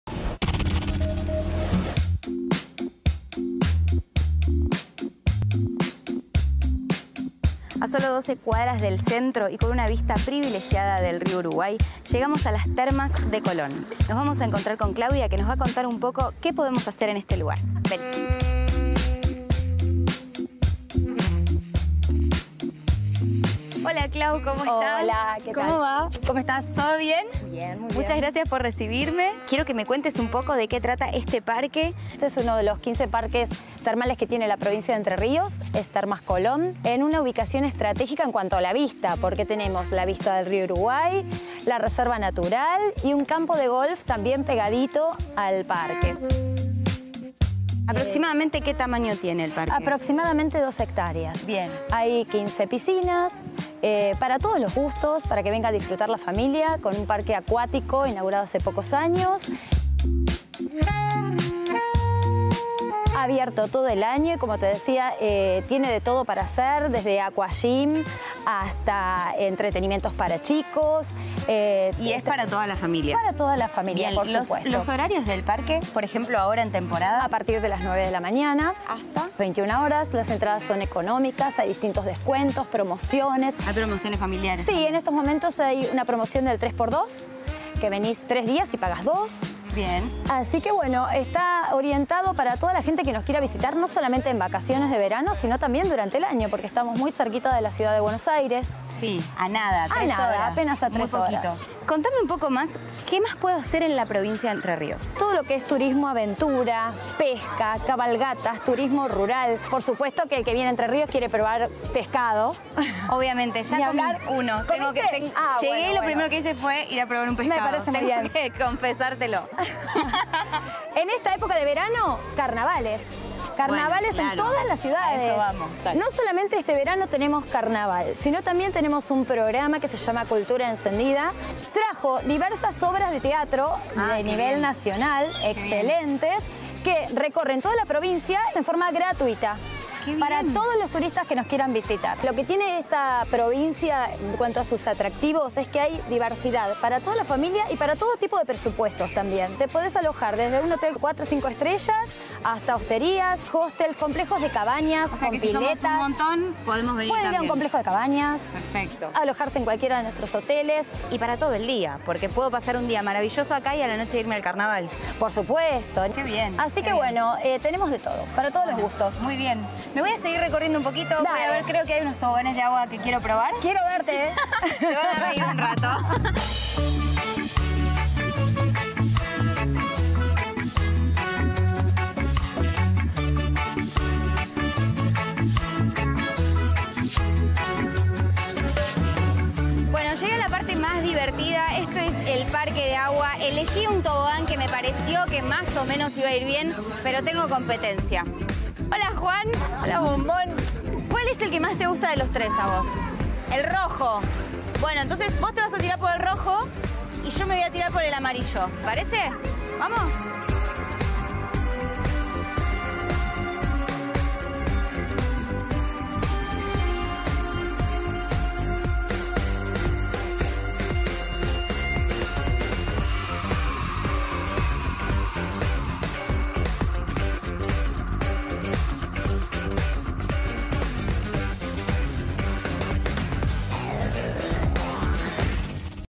Clave Argentina: Termas de Colón
Termas Colón cuenta con 15 piscinas de agua termal con diferentes profundidades, algunas de ellas están al aire libre, otras cubiertas, y una ofrece hidromasajes. Recomendado para toda la familia. Ubicación privilegiada en cuanto a la vista, abierto todo el año.